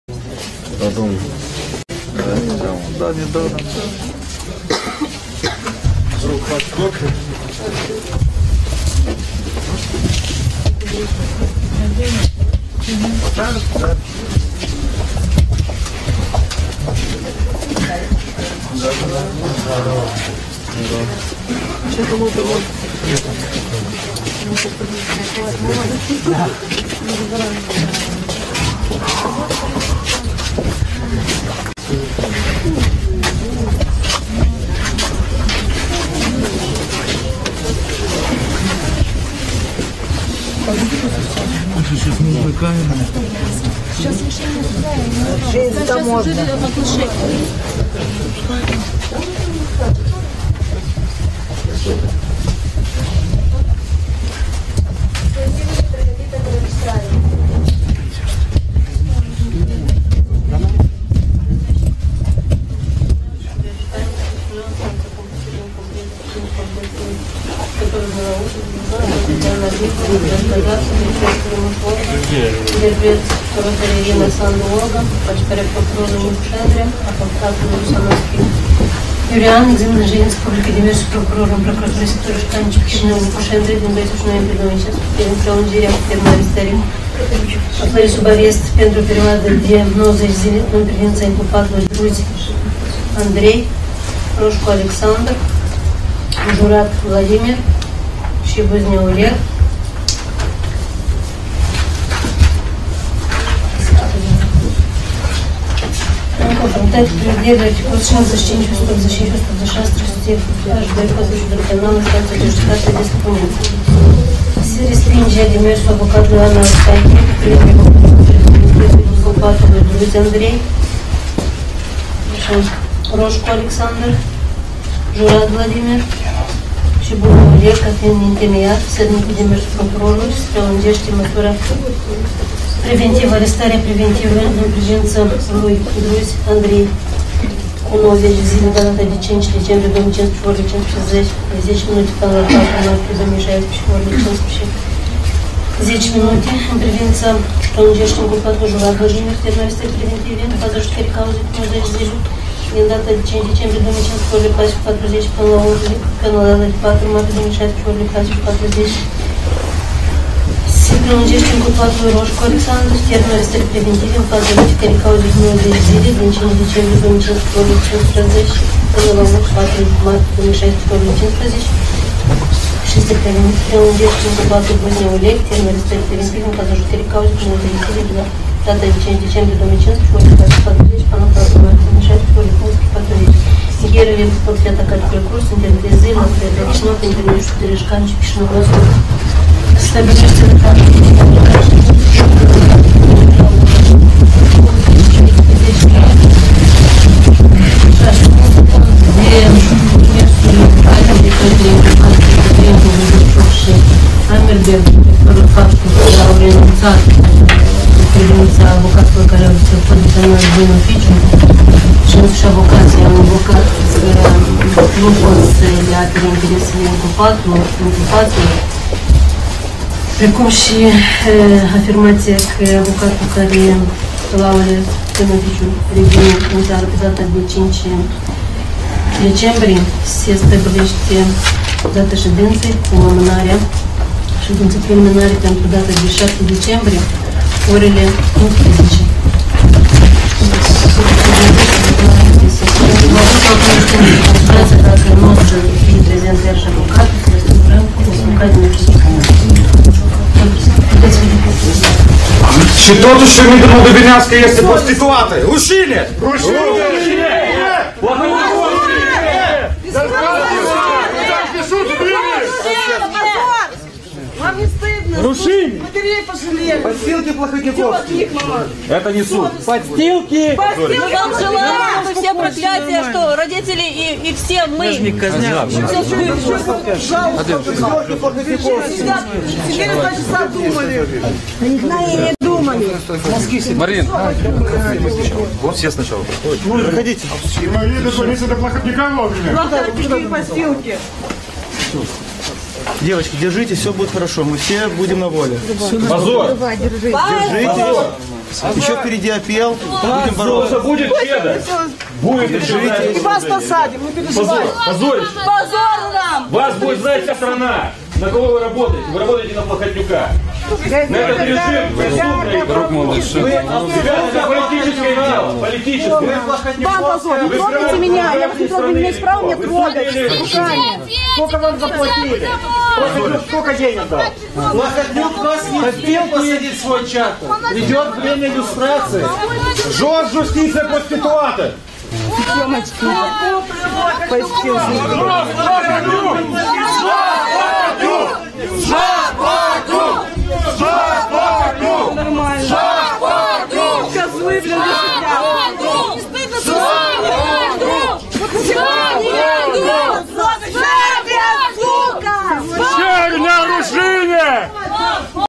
LIVE Оглашение приговора над политзаключенными «группы Петренко» 30.11.2015 "Omega Today" Moldova
Прямая трансляция новостного общественно-публицистического канала "OmegaToday" 24/7 /Молдова, Кишинев/